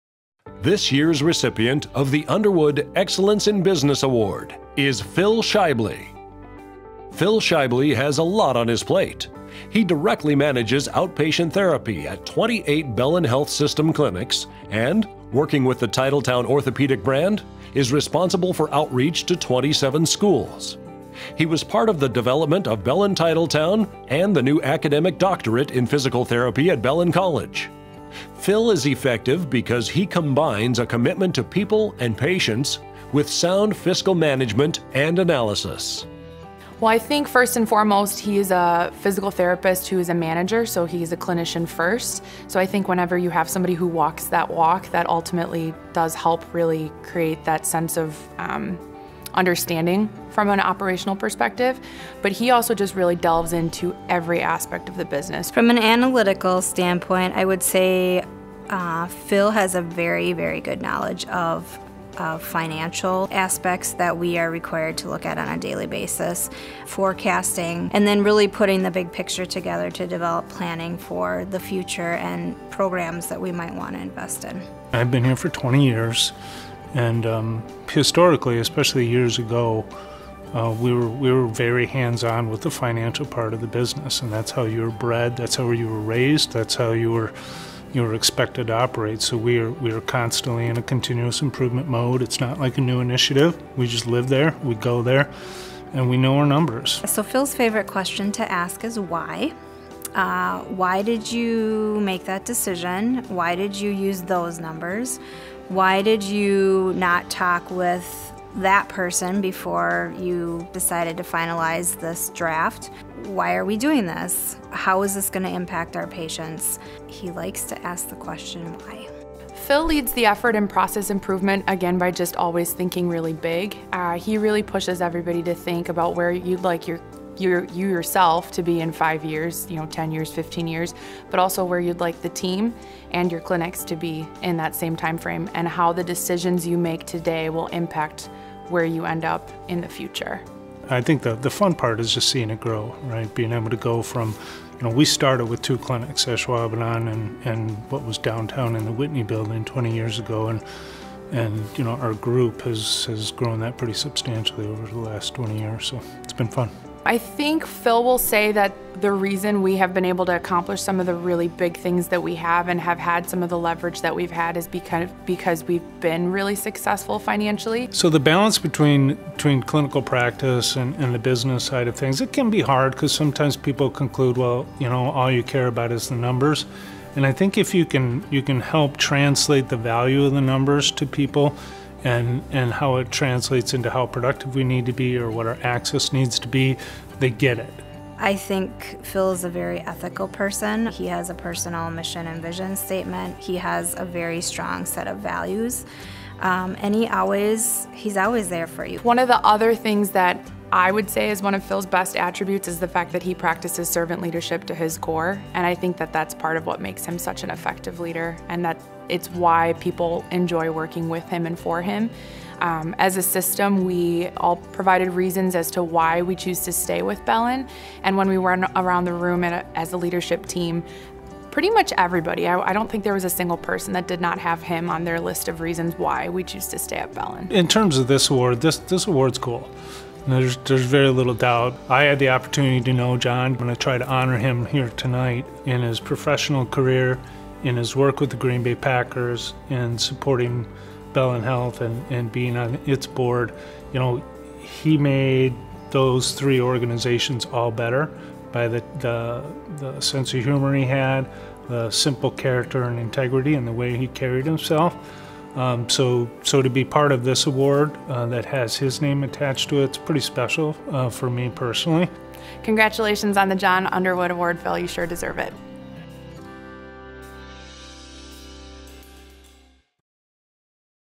2019 John R. Underwood Business Excellence Award
Phil Schaible is the recipient of the John R. Underwood Business Excellence Award. He’s being recognized for a commitment to accurate and objective business analytics, process improvement and integrity in business practices. Phil is a team leader for outpatient physical and occupational therapy, occ health services and athletic training services at Bellin Health.